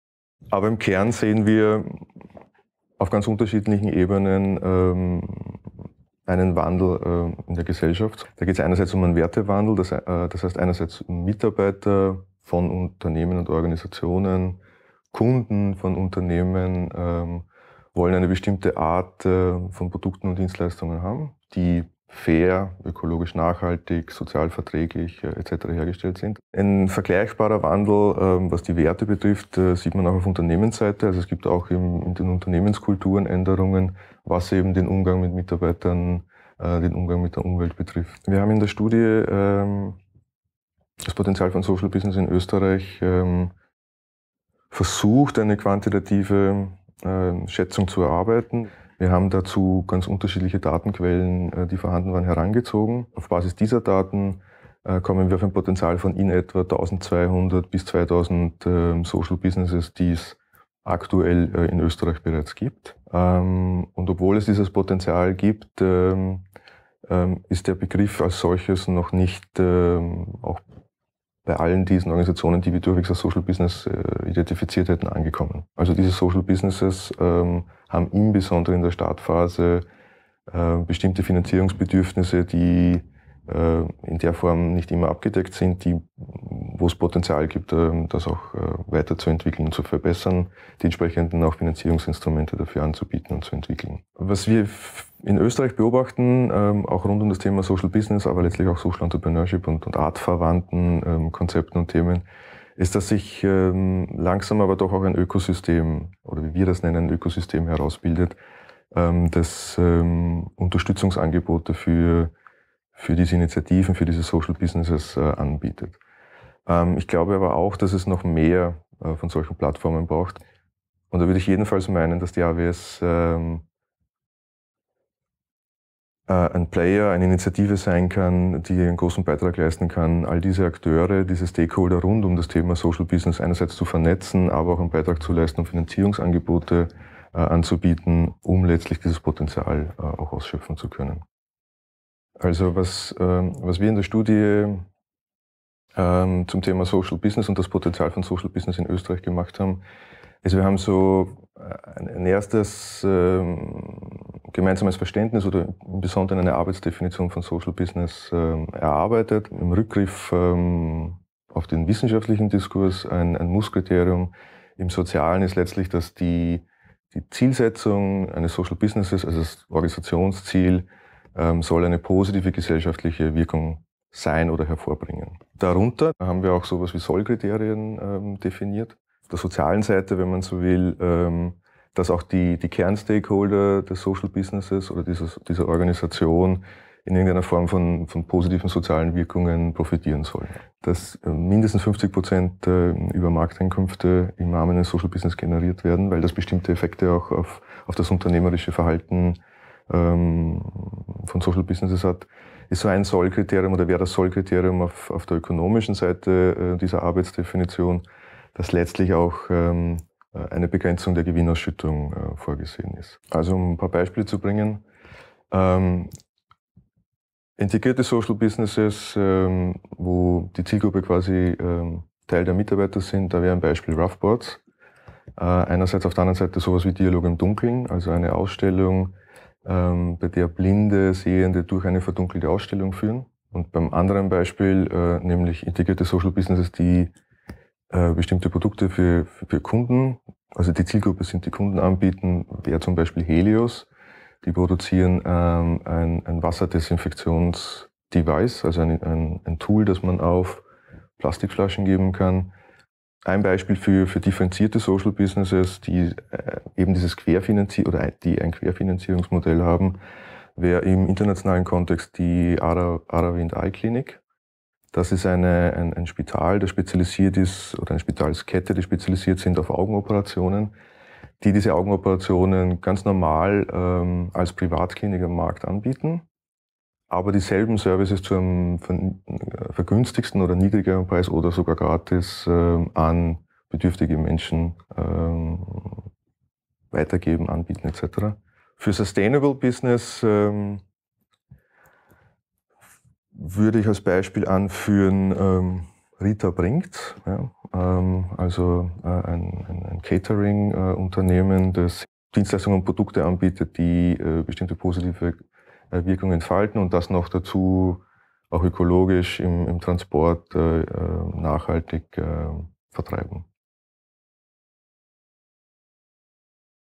aws Social Business - Reinhard Millner - Studienautor
Interview mit Reinhard Millner zum Thema Social Business in Österreich.